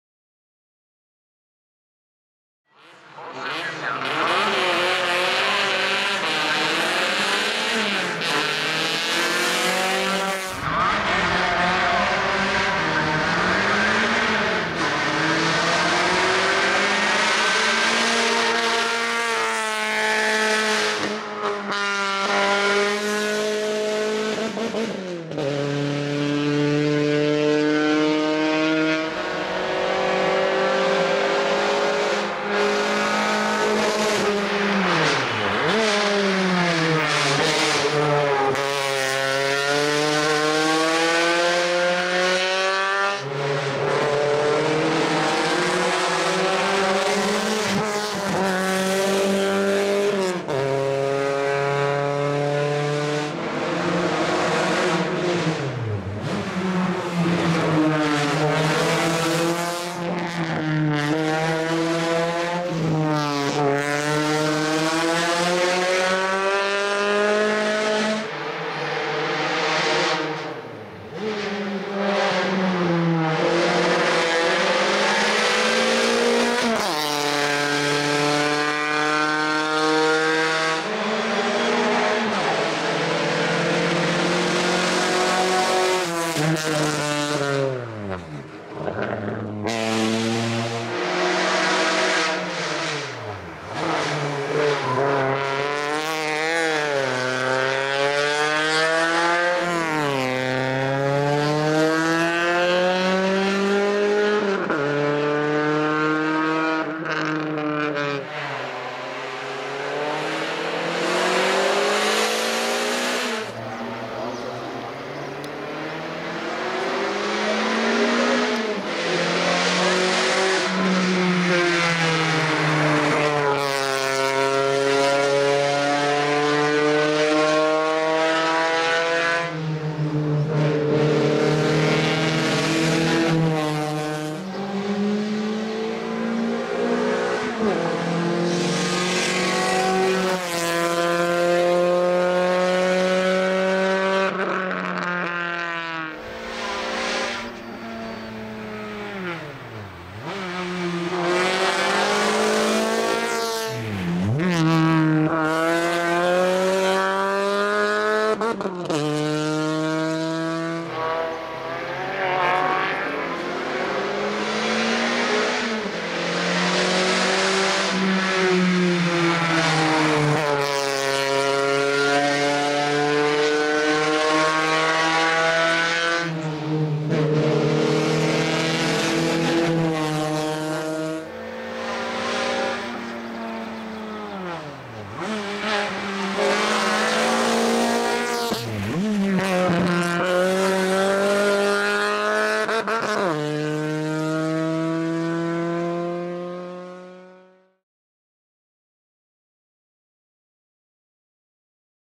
Volkswagen Scirocco Mk2 - Tomáš Karpíšek - XXXX. Zámecký vrch 2021 - Náměšť nad Oslavou
Tomáš Karpíšek si s vozem Volkswagen Scirocco Mk2 dojel pro osmé místo ve třídě HA1-2000 v závodech do vrchu XXXX. Zámecký vrch 2021 v Náměšti nad Oslavou.